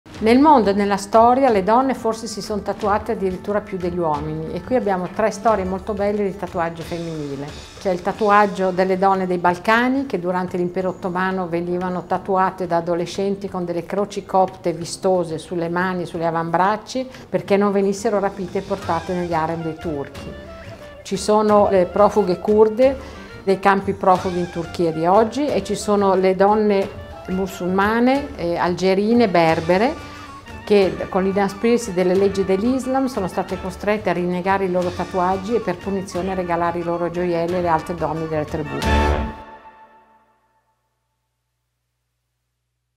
Tatuaggio. Storie dal Mediterraneo - Storie sul tatuaggio femminile
«Nel mondo e nella Storia le donne si sono tatuate forse più degli uomini».

Luisa Gnecchi Ruscone, co-curatrice di “Tatuaggio. Storie dal Mediterraneo” parte da qui per raccontarci una delle sezioni più affascinanti della mostra, dedicata ai significati del tatuaggio femminile tra le popolazioni balcaniche e nord-africane.

👉🏽 Ascolta il suo racconto per scoprire di più!

Tatuaggio. Storie dal Mediterraneo
📆 Fino al 28 luglio
📍 MUDEC, Milano

Video: Studio Spucches